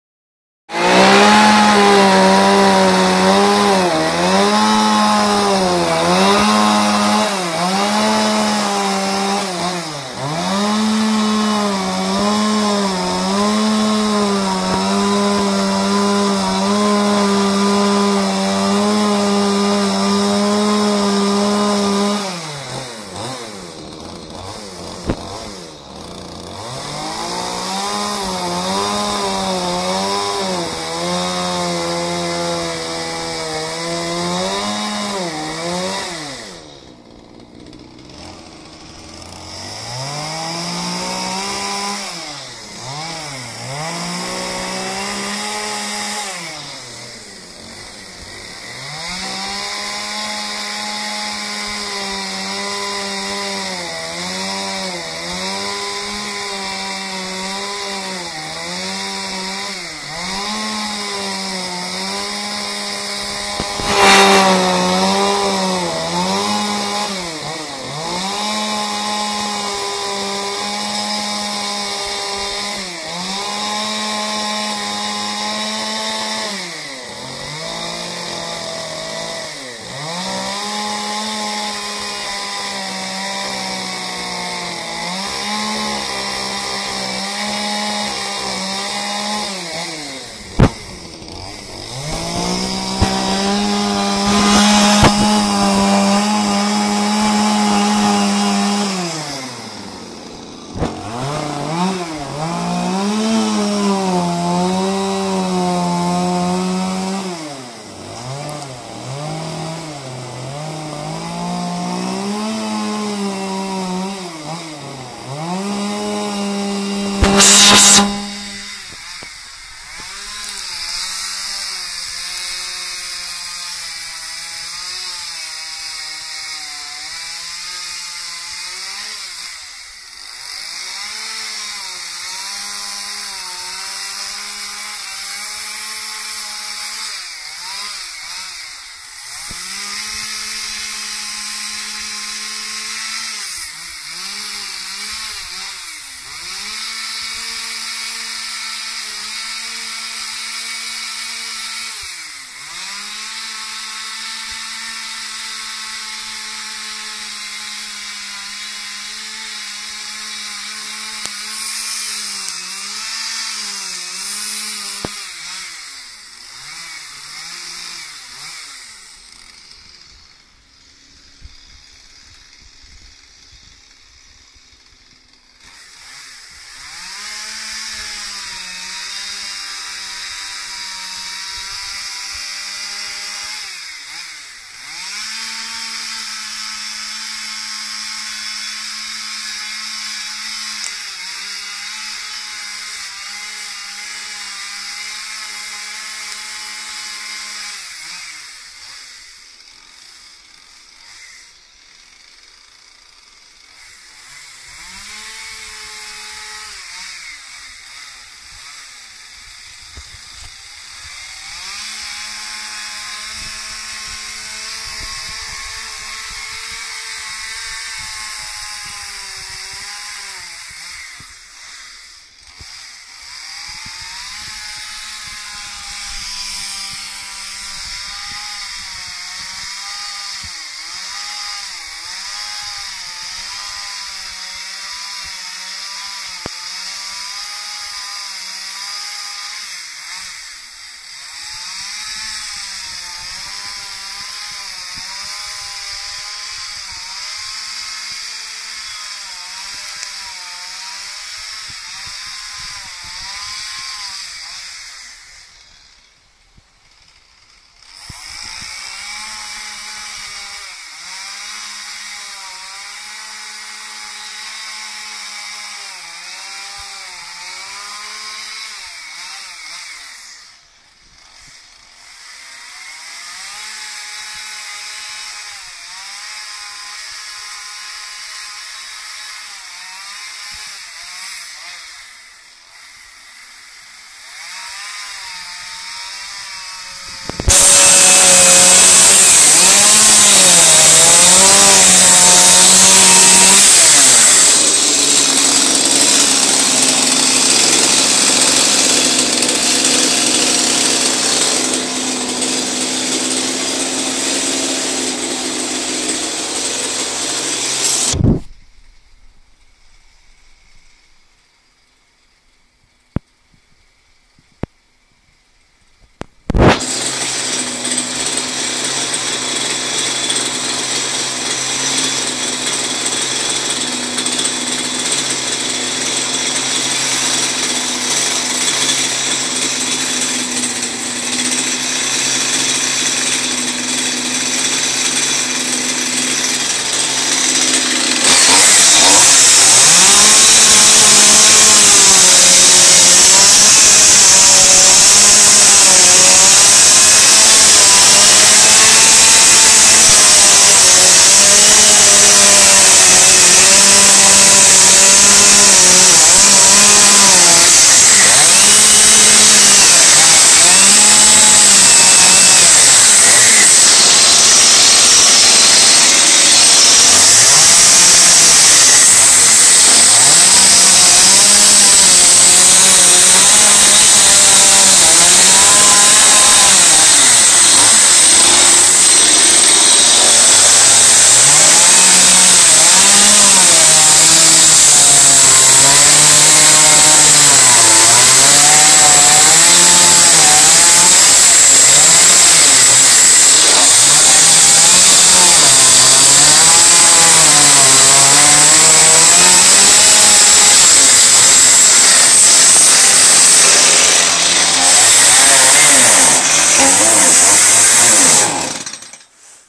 Chainsaw chicken carving
Chainsaw Sculptor Matthew Crabb carving a chicken stood on sacks from English Oak using a Stihl 181 with carving bar.